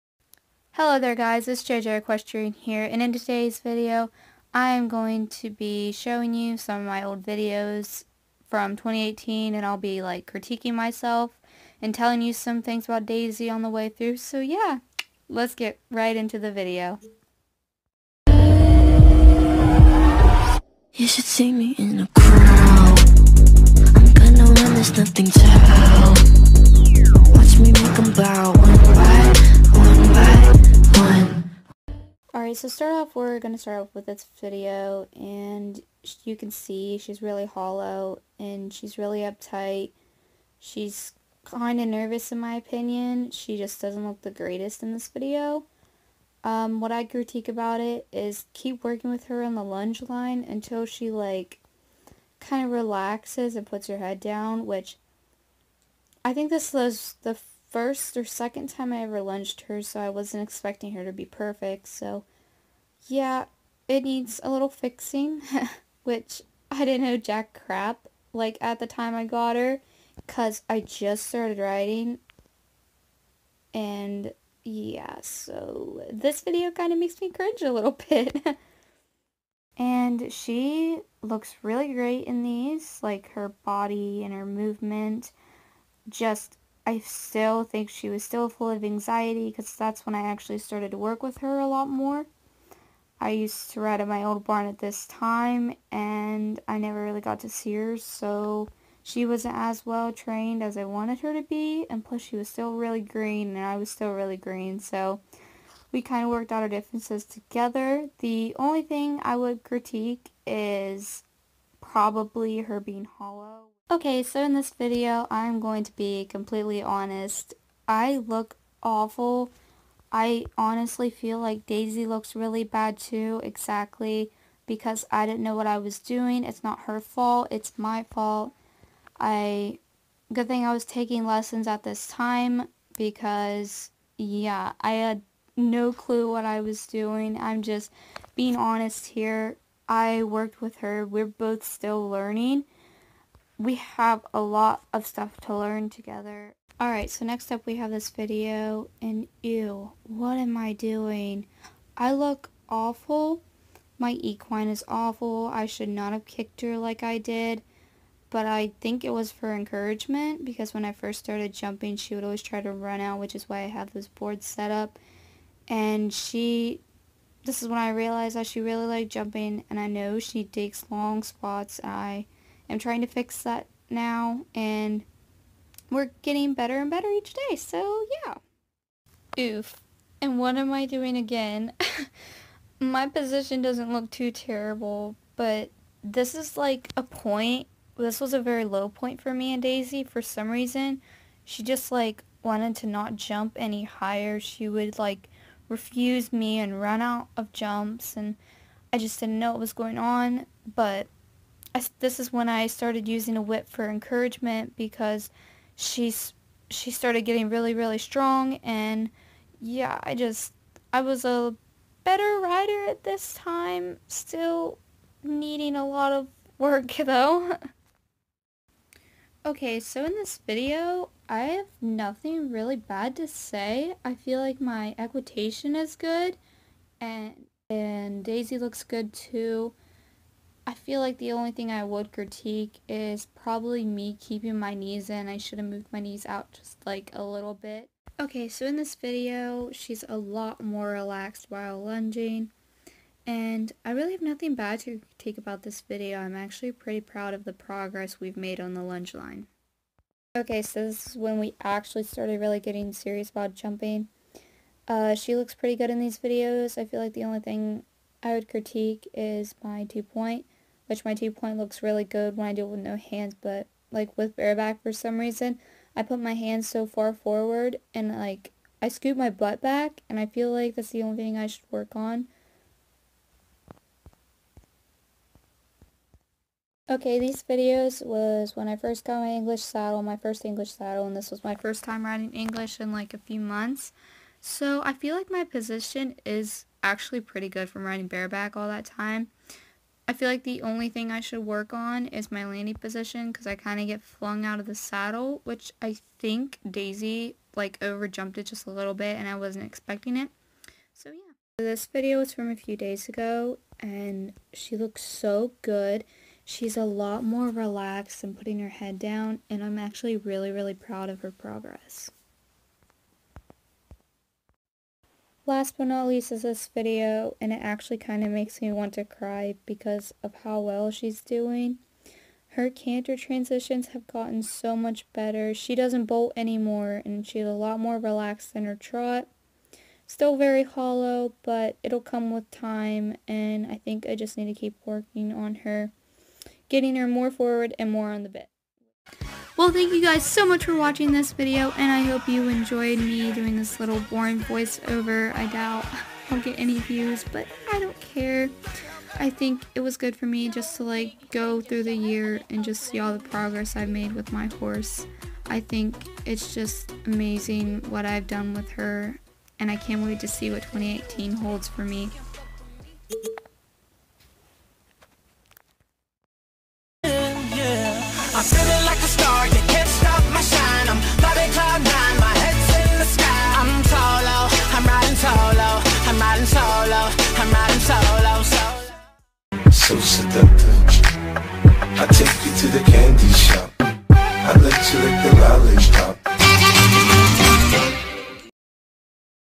2018 VOICEOVER 💞| JJ EQUESTRIAN
Hey y'all it's @jj.equestrian Instagram here and I'm officially starting my YouTube channel!! 💞
I thought this video was good for me to make 🙈😍
I feel so bad for my horse because she has to put up with me 😂
I'll see you in my next video BYEEEE 👋💞💥😉🤩